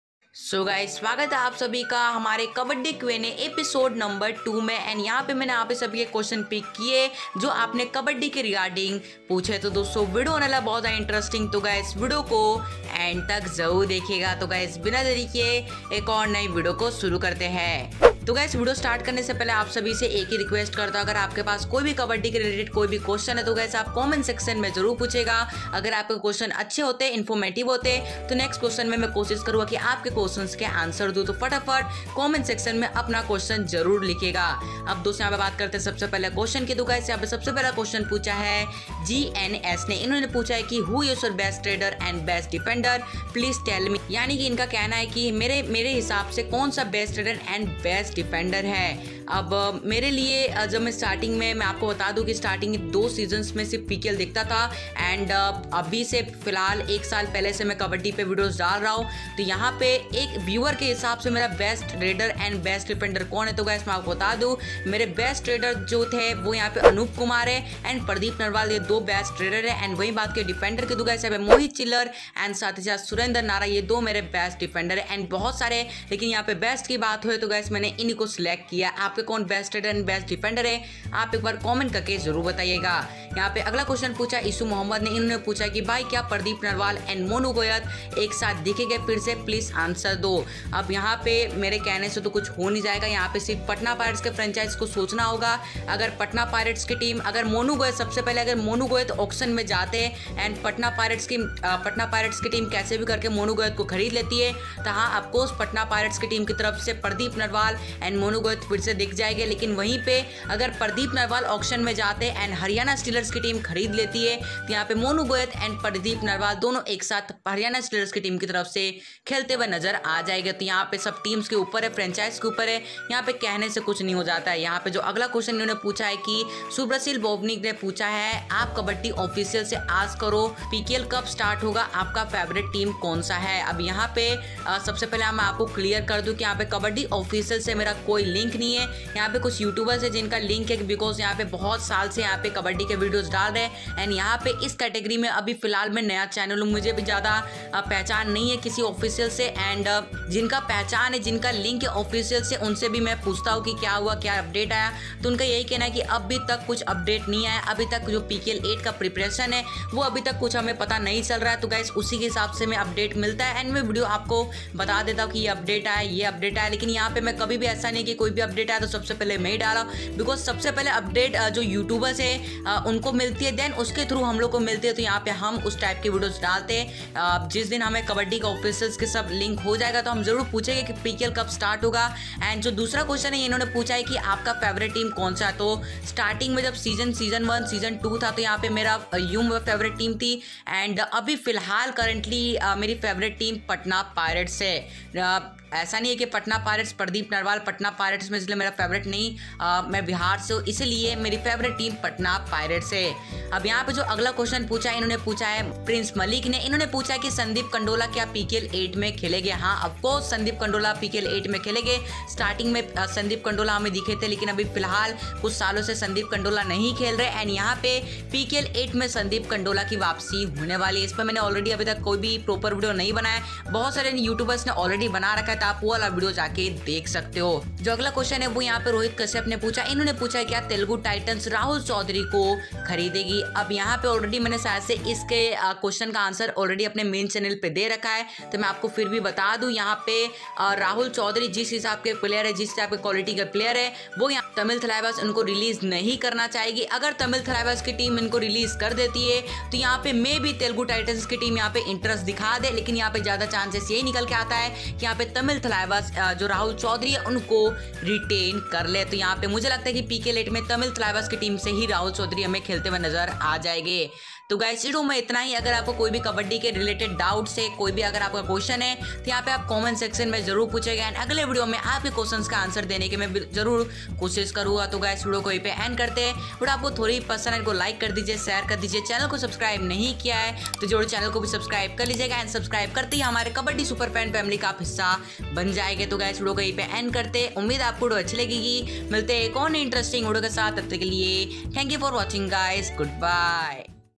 Sandeep Kandola Back in Pro Kabaddi 2021 | Pardeep & Monu in Same Team | Kabaddi SuperFans
HASTAGS
➤ Keep Supporting
➤ Love You